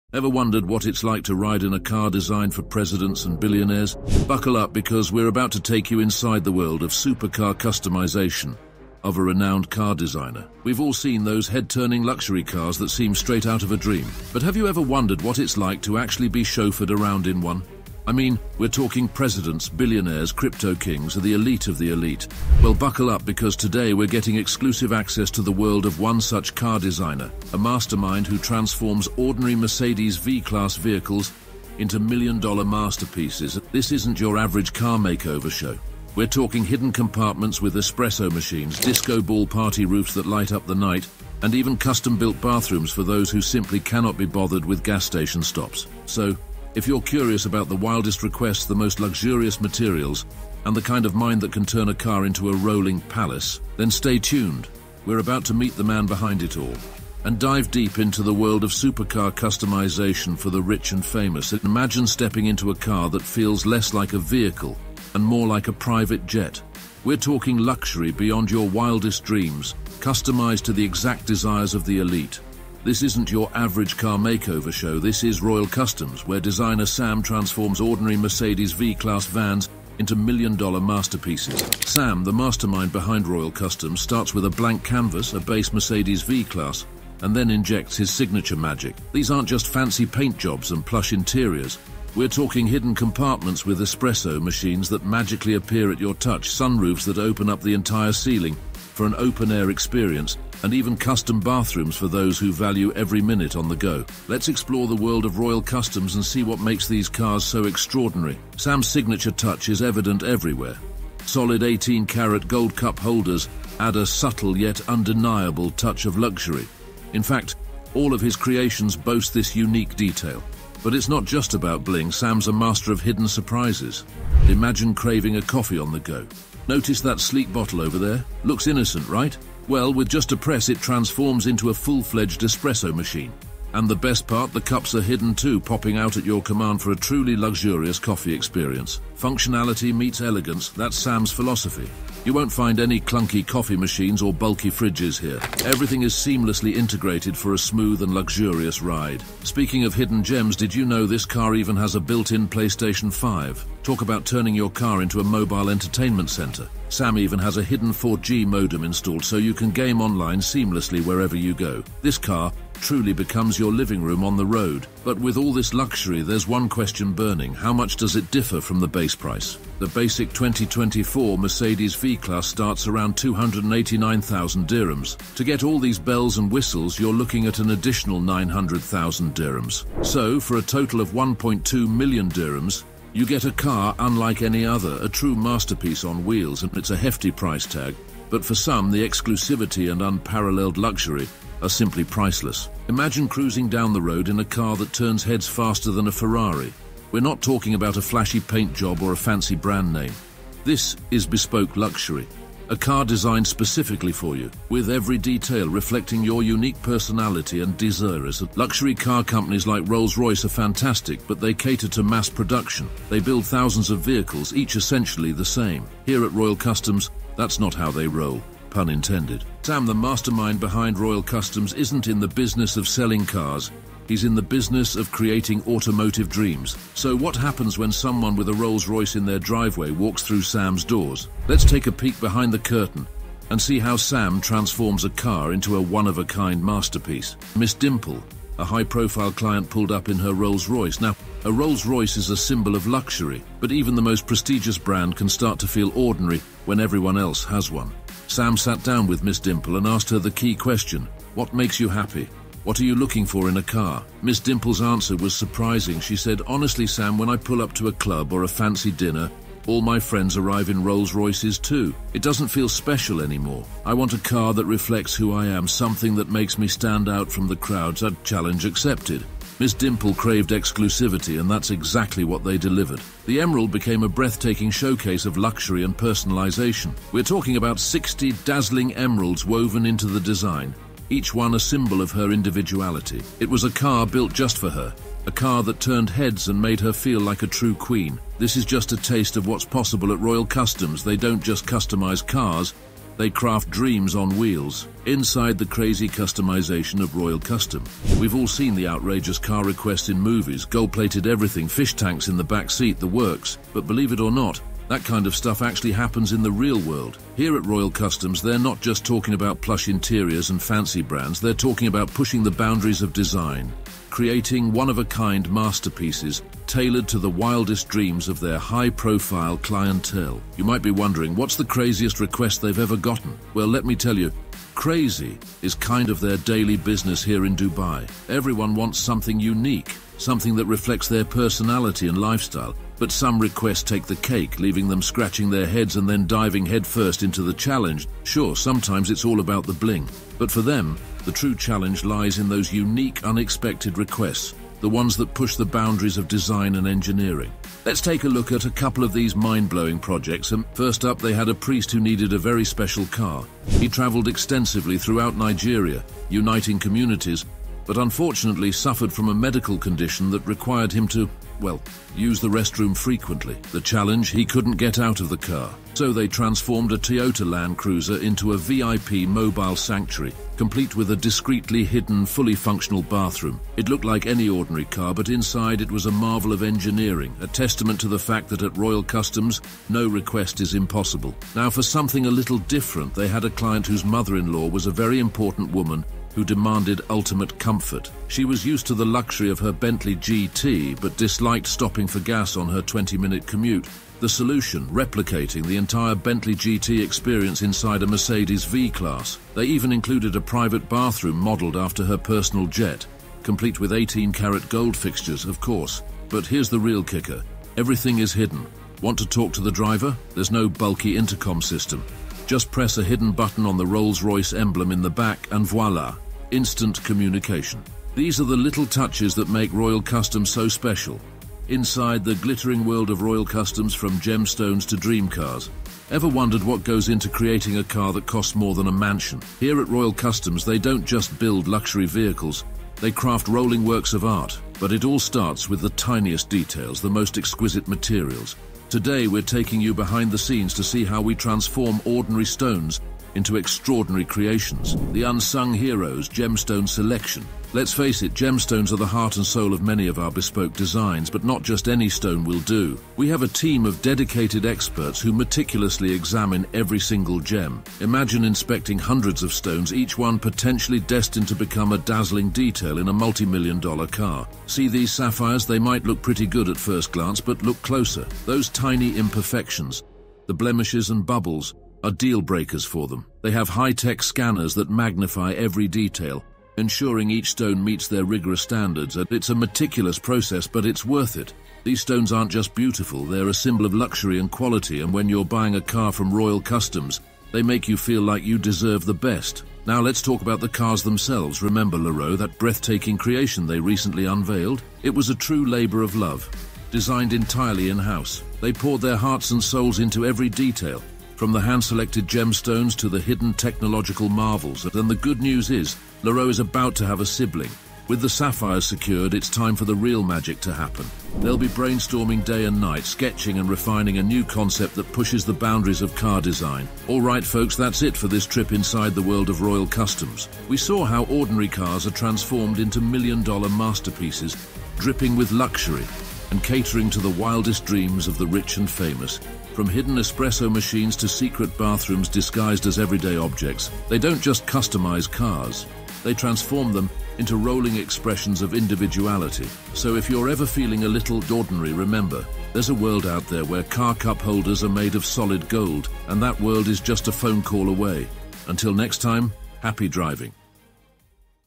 Transforming Mercedes: Secrets of Car Designer Unveiled
Ever wondered what it's like to ride in a car designed for presidents and billionaires? This video takes you on an exclusive tour of Royal Customs, where ordinary Mercedes-Benz V-Class vans are transformed into million-dollar masterpieces.

Prepare to be amazed by:

Hidden compartments with espresso machines that appear at your touch
Disco ball party roofs that light up the night
Custom-built bathrooms for those who can't be bothered with gas station stops

We'll meet Sam, the mastermind behind Royal Customs, and discover:

His signature touches, like solid 18-karat gold cup holders
How he integrates hidden luxuries like PlayStation 5s and 4G modems
The jaw-dropping price tag of these customized supercars

But Royal Customs isn't just about bling. They create bespoke luxury, crafting cars that reflect your unique personality.

See how they transformed a Rolls Royce owner's car into "The Emerald," a one-of-a-kind masterpiece adorned with 60 dazzling emeralds.

This is just a taste of what's possible at Royal Customs. Click play to see more!